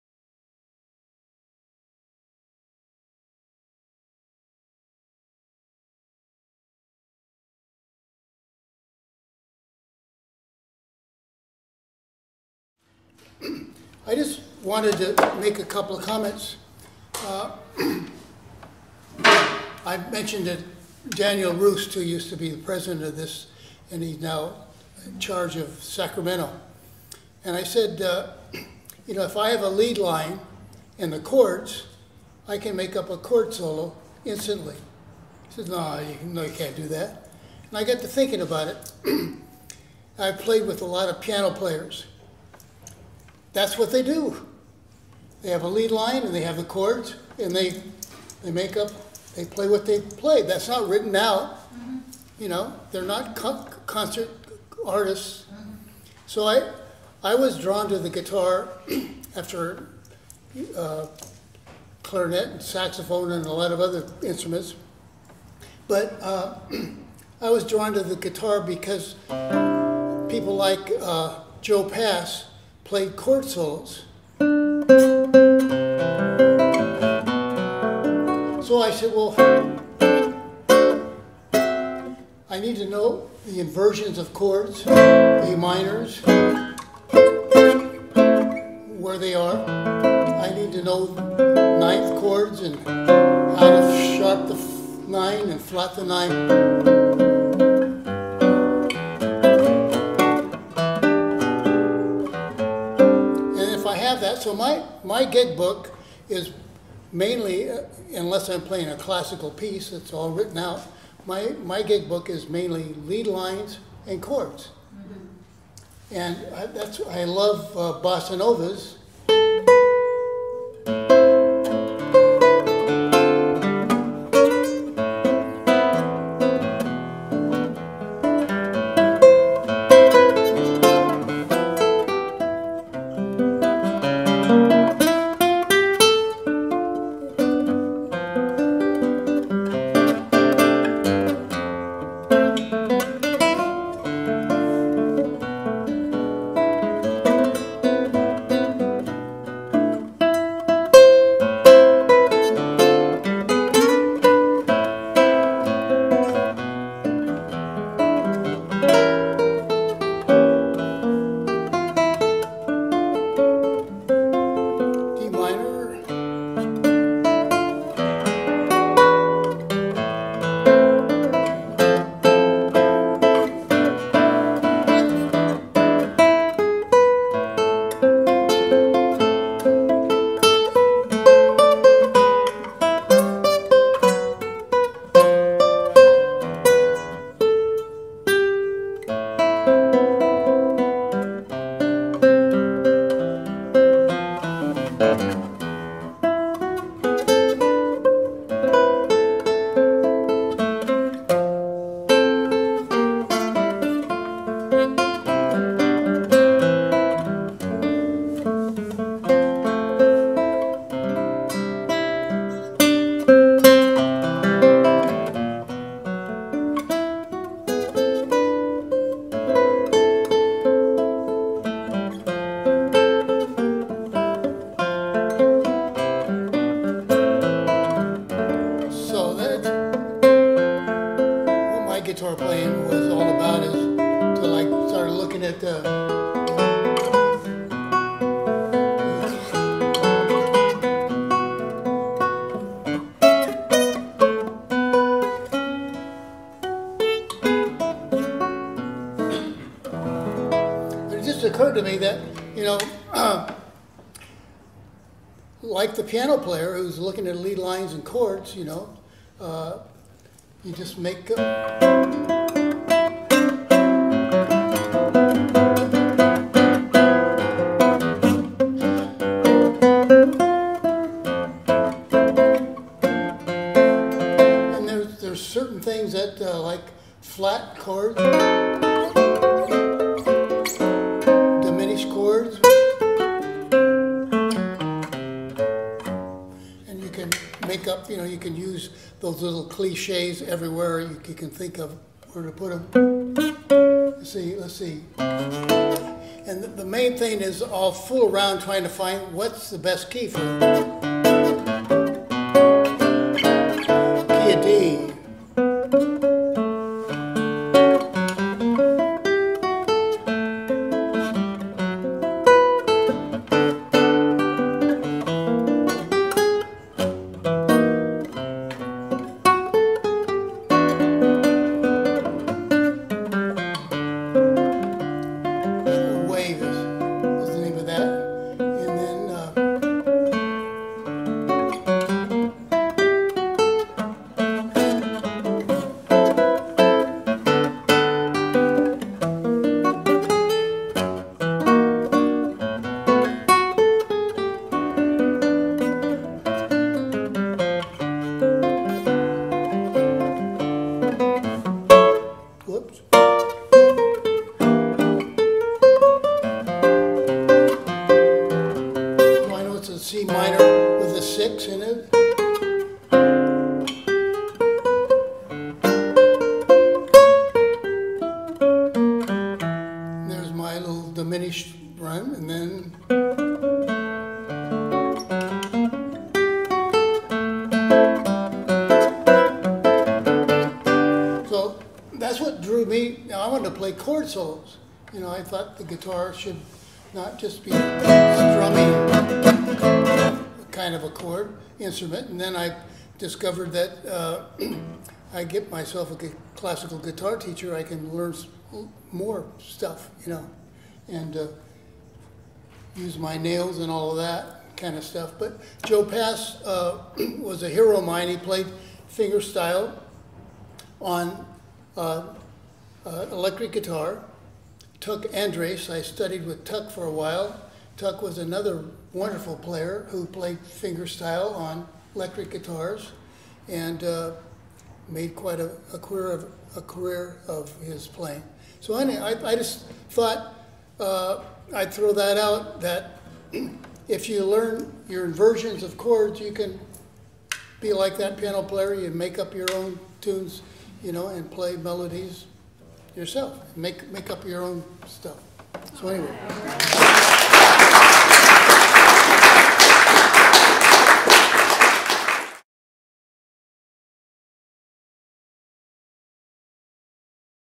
Jerry Snyder - Improvisation on the guitar
Jerry Snyder - Improvisation on the guitar, SBGS Open Mic February 2020